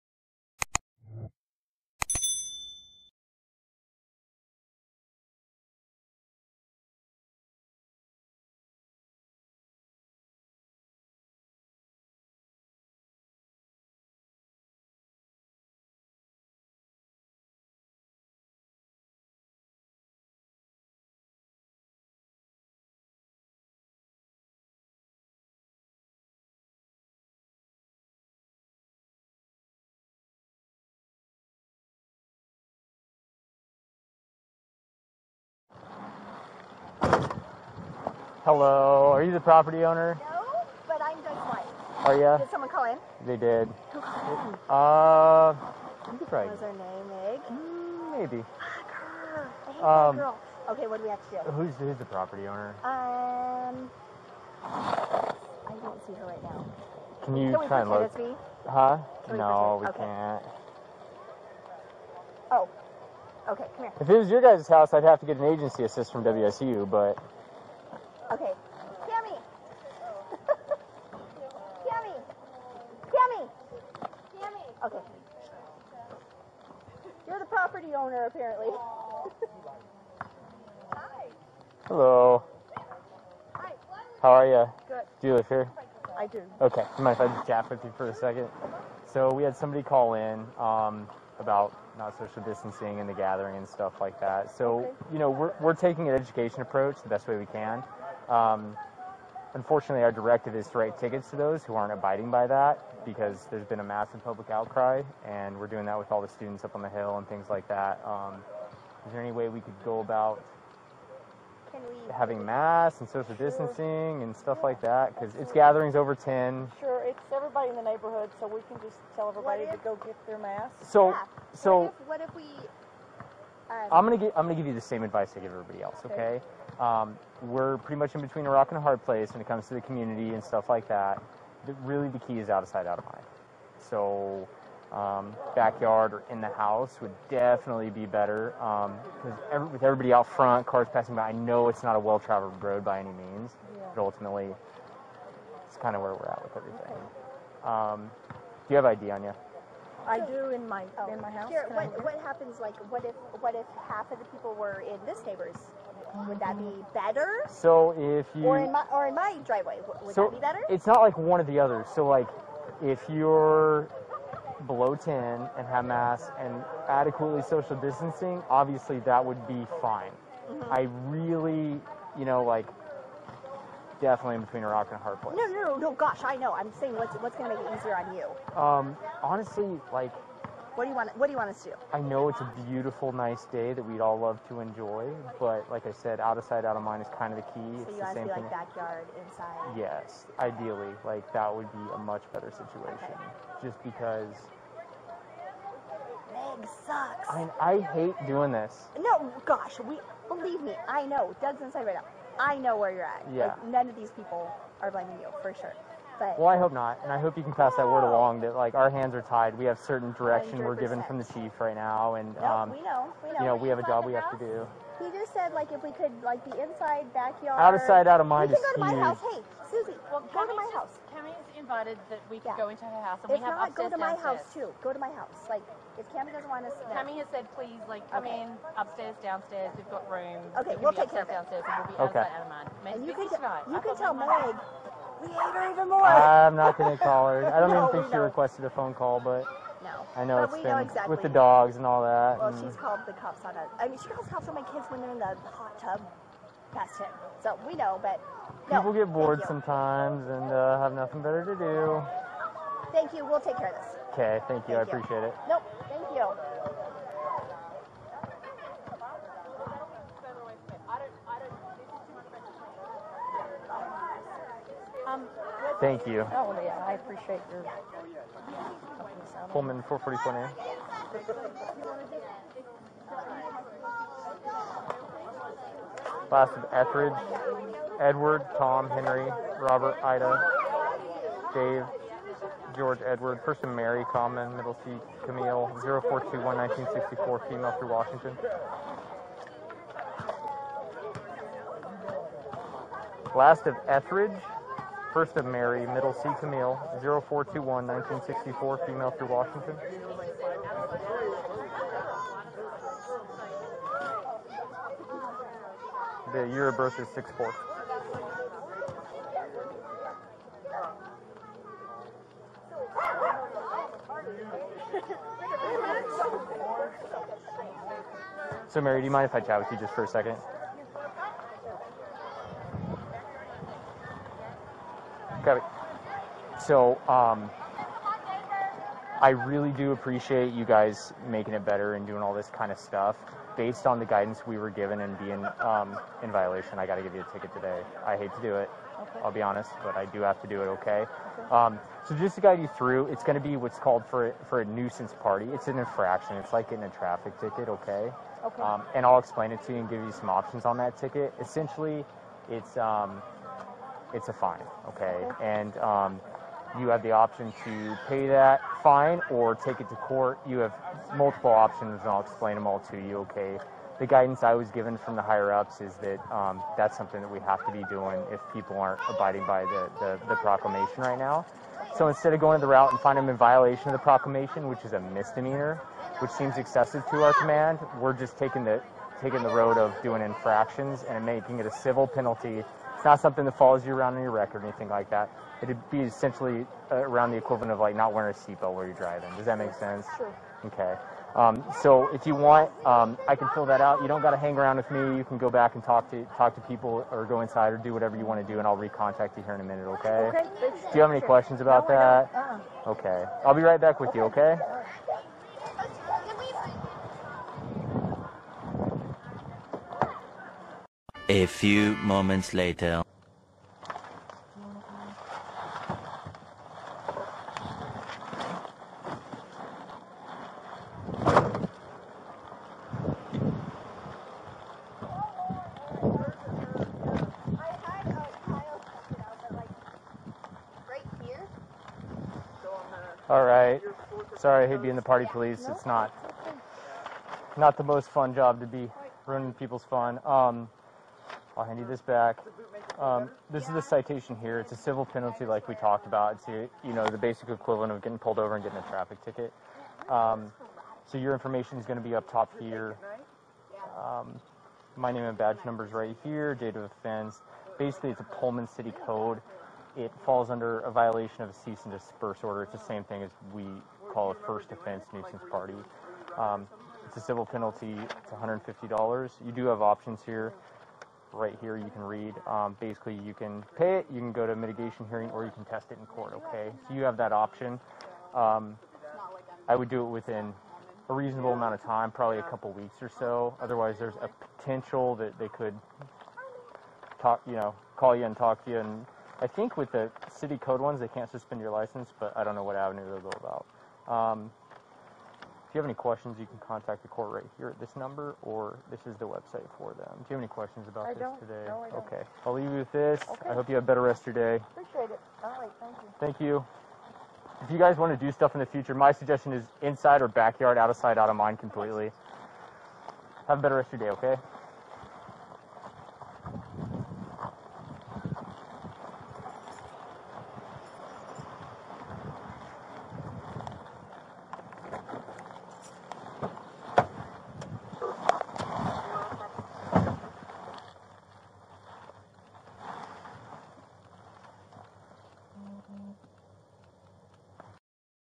Bodycam video shows Pullman officer recommending partygoers stay "out of sight, out of mind"...
Body camera video from the Pullman Police Department shows an officer recommending that people having a party in August remain indoors or in their backyard rather than out in the open.

Whitman County has experienced a spike in COVID-19 cases since Washington State University students returned to Pullman, in part due to large gatherings and parties.

In response, Pullman Police Chief Gary Jenkins announced in late August that the department would cite people who have parties or gatherings with more than 10 people.

The tickets are for "nuisance party infractions," according to the Pullman Police Department. "Nuisance party" is a local ordinance that prohibits anyone from hosting or allowing a party.

But body camera video obtained by KREM through a records request shows an officer giving partygoers advice abut how to skirt the ordinance.

The video shows a Pullman officer responding to a large gathering at the home of WSU women's basketball coach Kamie Ethridge on Aug. 29, 2020. It appears that more than a dozen people, who did not wear masks, were in attendance. An outdoor tent and what appears to be cups of alcohol are visible in the video.

In the video, the officer says that someone called in about the gathering in front of the home and lack of social distancing in place.

“We’re taking an education approach the best way we can. Unfortunately, our directive is to write tickets to those who aren’t abiding by that because there’s been a massive public outcry because we’re doing that with all of the students on the hill," the officer said, likely referring to parties taking place on WSU's College Hill.

The officer asks if people at the party can wear masks before encouraging them to hold gatherings inside of a home or in a yard. He emphasizes multiple times in the video that partygoers should stay "out of sight, out of mind."

“I’m going to give you the same advice I give everybody else, okay? We’re pretty much in between a rock and a hard place when it comes to the community and stuff like that. Really the key is out of sight, out of mind," the officer said.

In a statement sent to KREM on Tuesday, Chief Jenkins said the police department has been randomly viewing body camera footage of nuisance party ordinance enforcement and has not seen "messaging similar to the incident in question." He called the messaging in the video "inappropriate."

"After viewing the video, I made it clear to all my staff that messaging should be to gain compliance with the nuisance party ordinance to reduce the risk of COVID-19 spread in the community," Jenkins wrote in part.

The officer did give Ethridge a ticket for the party hosted on her property. In a statement released by WSU Athletics, she apologized for the violation of coronavirus guidelines:

“I would like to apologize for the violation of our local social distancing guidelines.  There was a gathering held on my driveway, of which I did not host. But as the guidelines state, the owner of the property is responsible for maintaining proper social distancing and mask wearing for all that are in attendance. As this did occur on my property, I take full responsibility. I appreciate the hard work being done by our Whitman County Health officials to keep our community as safe as possible during this pandemic. As a role model in our community, I will work to set a better example.”

***This video is intended for educational and informational purposes only. I do not recommend anyone attempt to reenact any part of this video or create hate from what you see in this video or any other videos***